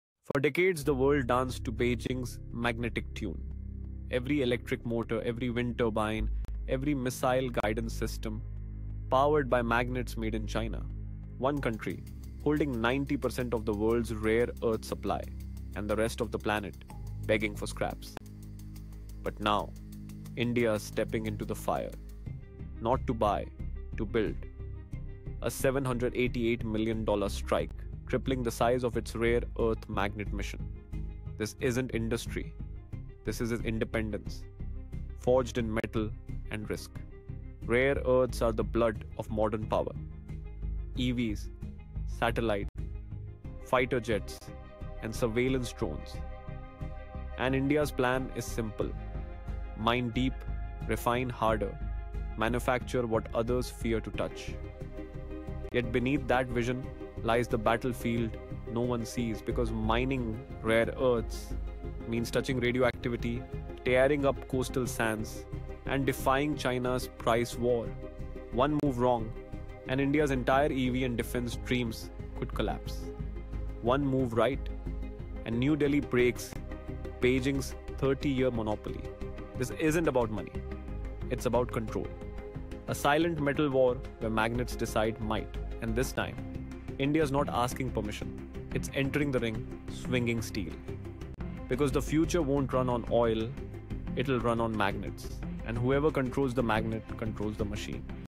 𝗧𝗵𝗲 𝗠𝗮𝗴𝗻𝗲𝘁 𝗪𝗮𝗿: 𝗜𝗻𝗱𝗶𝗮’𝘀 𝗦𝘁𝗿𝗮𝘁𝗲𝗴𝗶𝗰 𝗘𝘅𝗽𝗹𝗼𝗶𝘁𝗮𝘁𝗶𝗼𝗻 . . . . . . .
India's Bold Move in the Global Rare Earth Magnet Market

Sources -

For decades, the world has relied on China's dominance in rare earth materials, controlling 90% of the global supply. However, I am excited to share that India is stepping up with a bold $788 million initiative to build its own rare earth magnet industry. This mission is not just about industry; it’s about achieving independence and breaking China’s long-standing monopoly. We aim to mine deeper, refine harder, and manufacture what others fear to touch, despite the risks involved. I urge you to recognize the significance of this silent metal war, as the future will depend on our ability to control these vital resources.